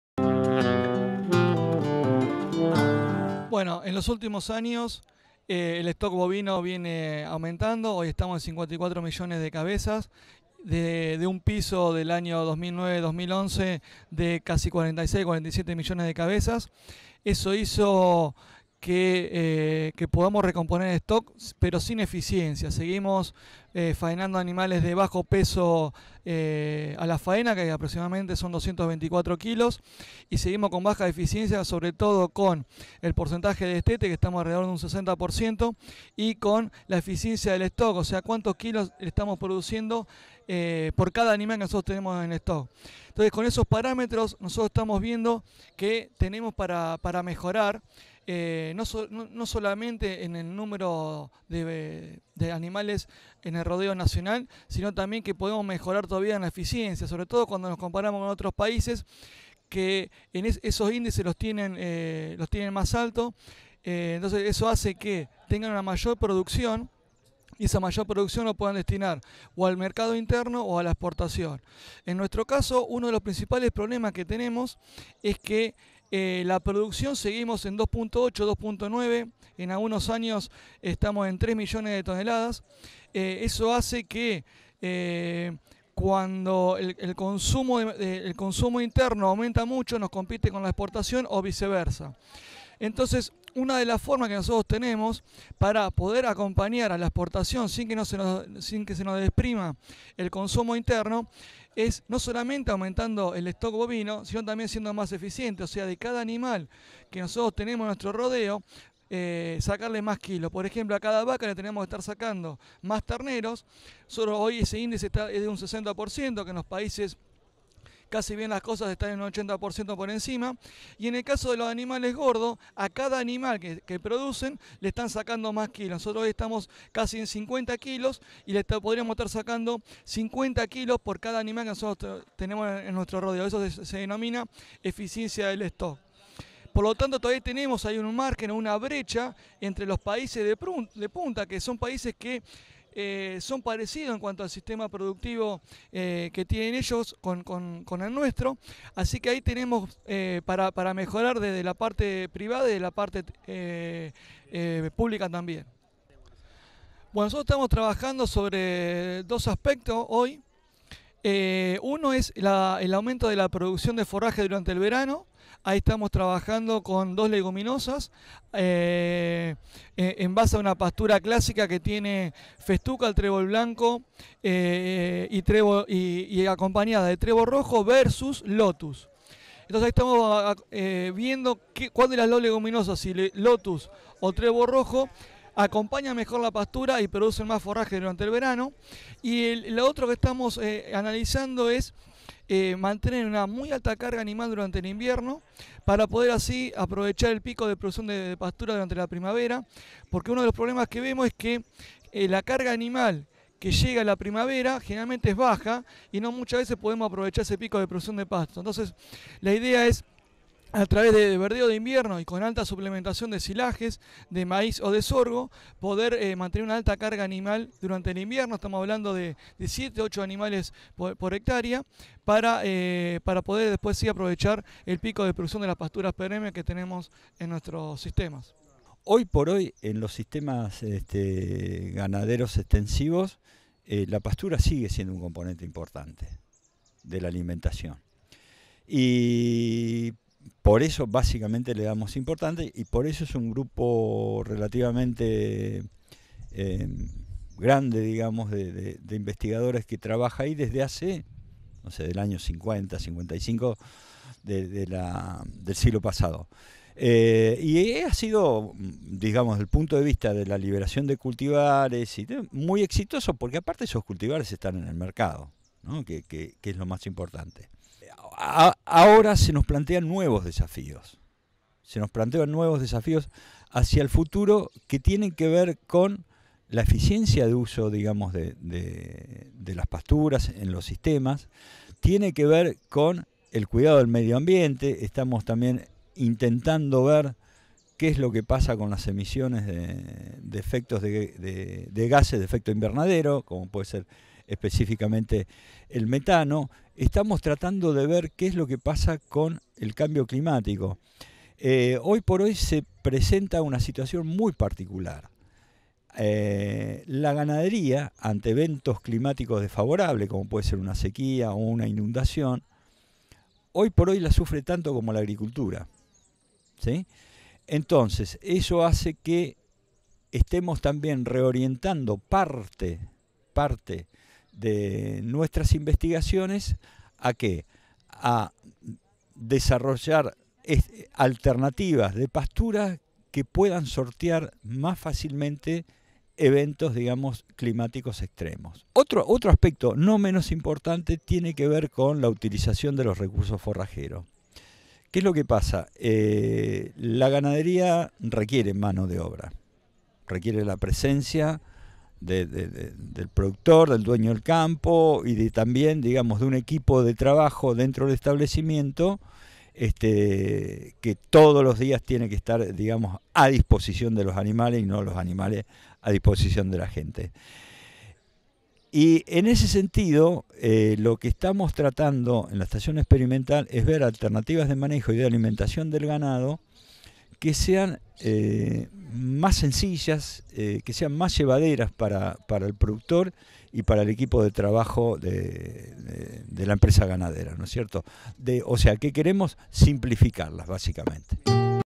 Ganadería y gestión forrajera
El crecimiento del stock ganadero en nuestra región en la última década trae nuevos desafíos, relacionados con la eficiencia, la calidad y, en el caso de la cría extensiva a campo, el aprovechamiento de forrajes. El grupo de investigación de INTA Pergamino marca una línea de trabajo en el tema de más de 60 años, y en esta entrevista nos comparten su enfoque y sus avances.